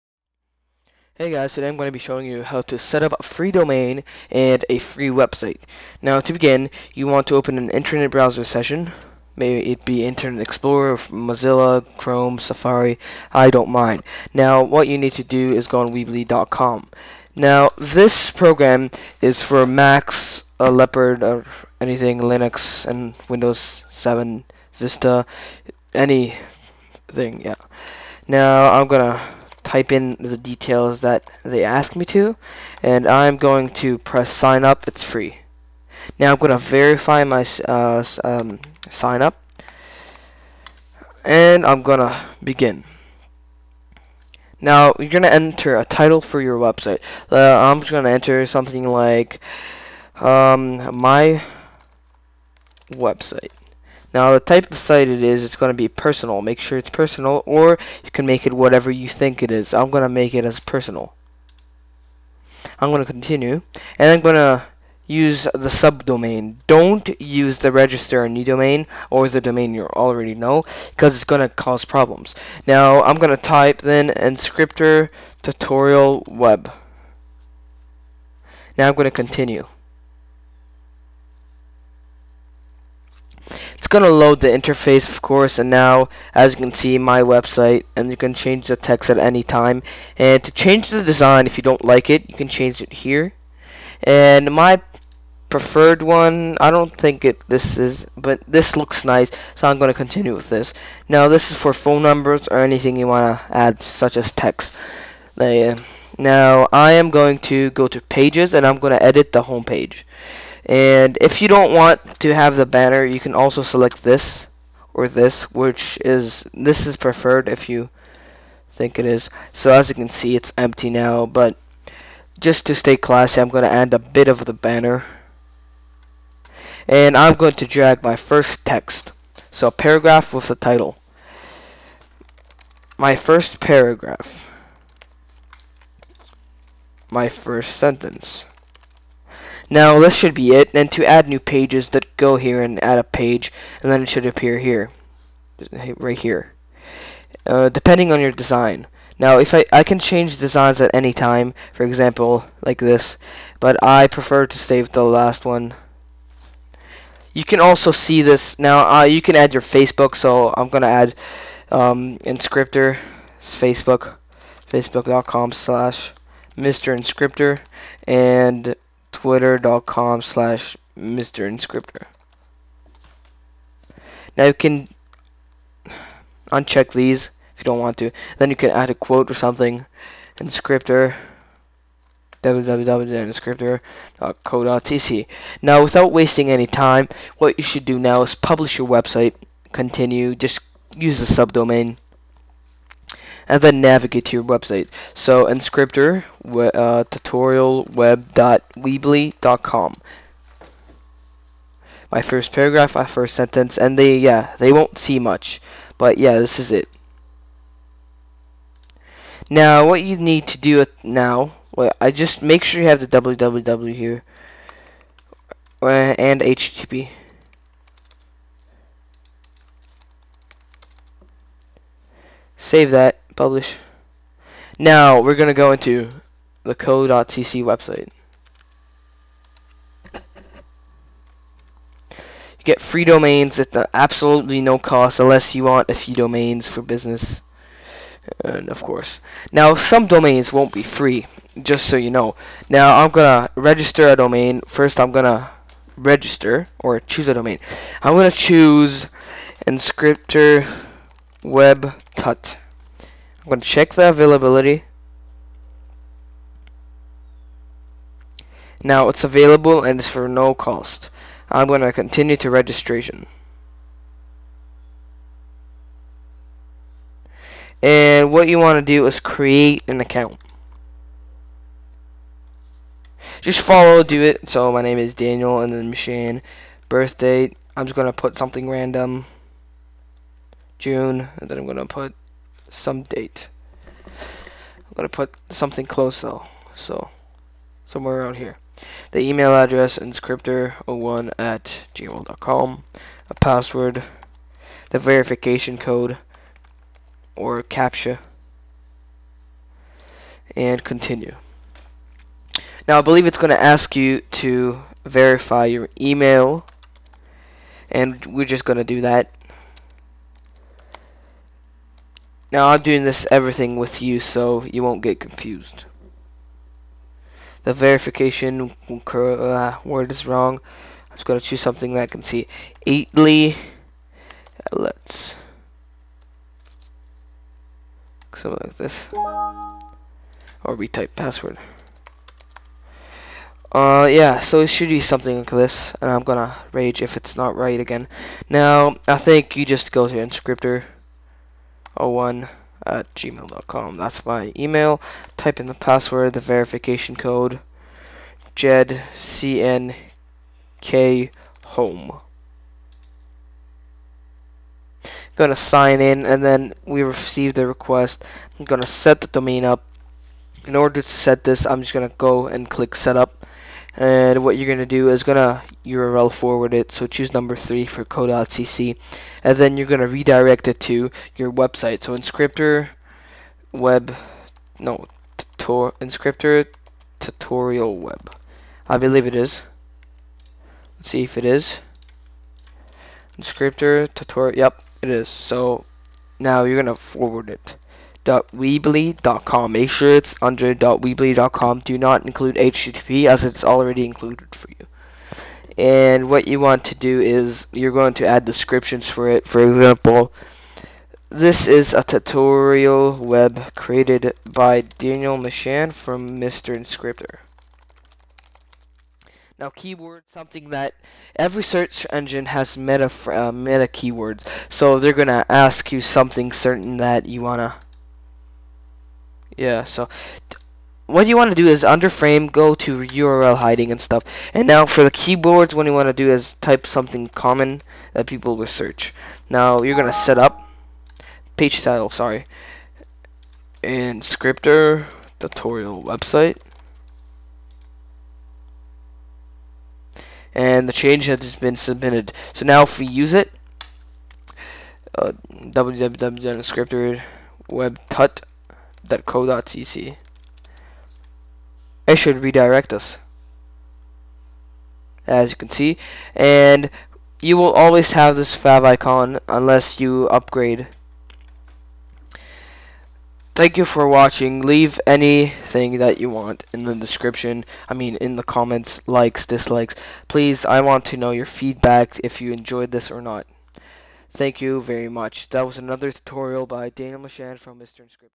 How to set up a FREE WEBSITE w/ FREE DOMAIN in minutes!
Join MrInscriptor as he helps you set up a free website at no cost with a free year-legth domain.